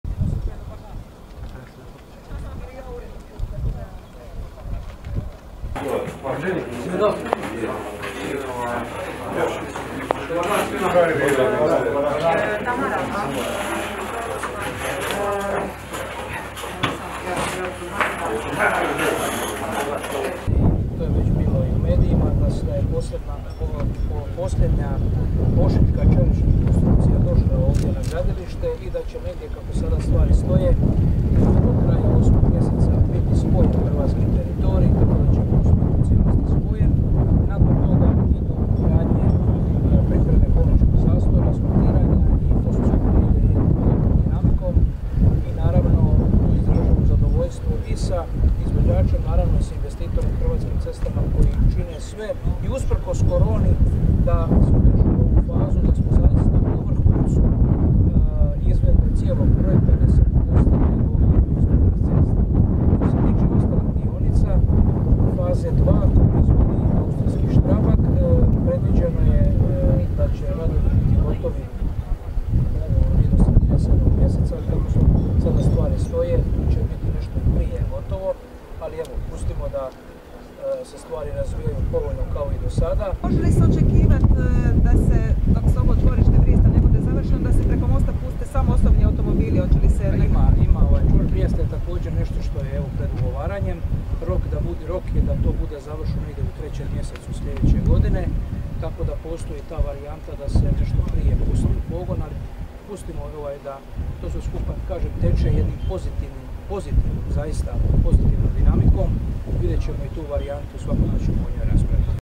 Ministar Oleg Butković obišao gradilište Pelješkog mosta
Komarna, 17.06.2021 - Ministar Oleg Butković obišao gradilište Pelješkog mosta. Snimio: Tonči Plazibat / CROPIX